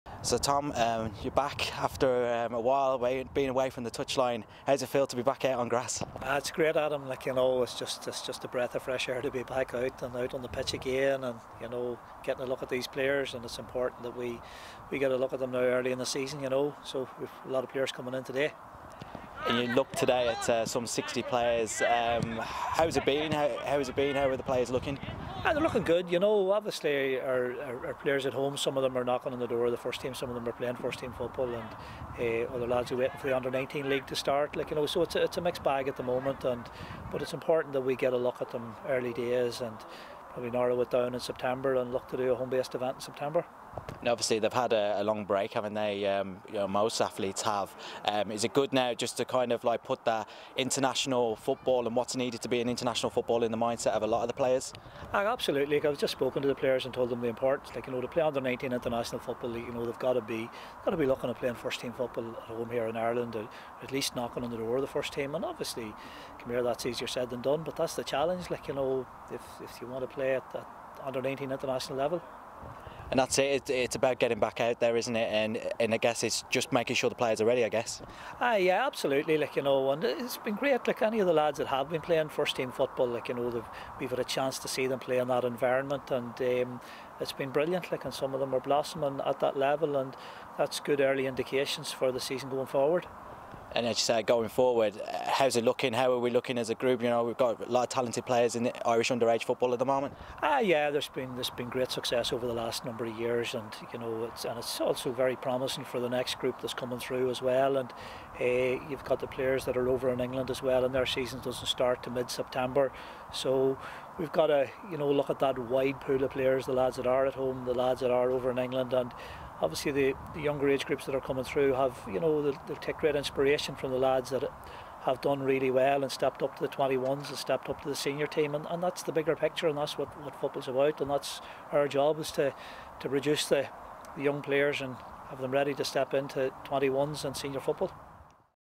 IRLU19 Head Coach Tom Mohan prepares for 2020/21 season
Tom Mohan took a close look at 60 home-based players as he weighs up his options for the forthcoming international campaign.